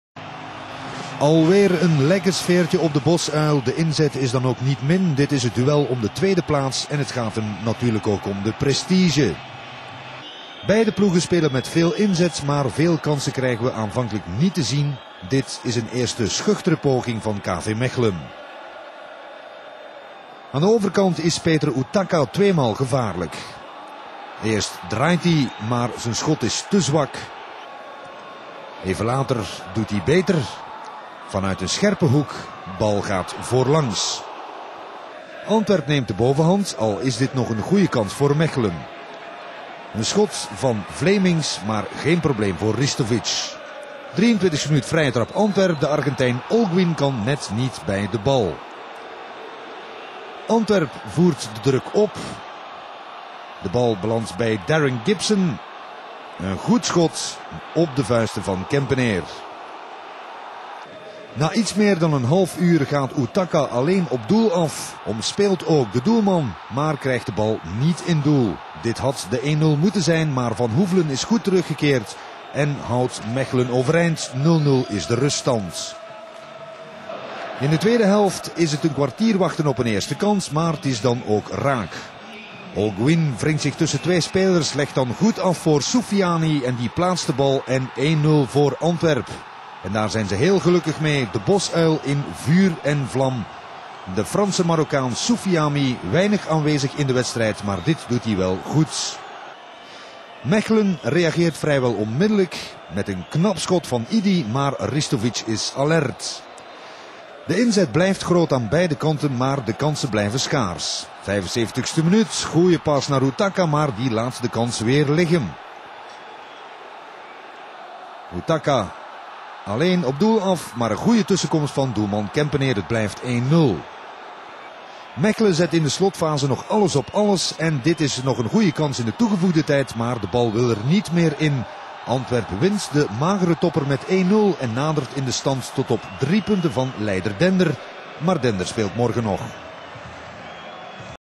20070310 | League | R.A.F.C. - Y.R. K.V. Mechelen | VRT
Matchday 25 - March 10th 2007
R. Antwerp F.C. - Y.R. K.V. Mechelen ~ 1-0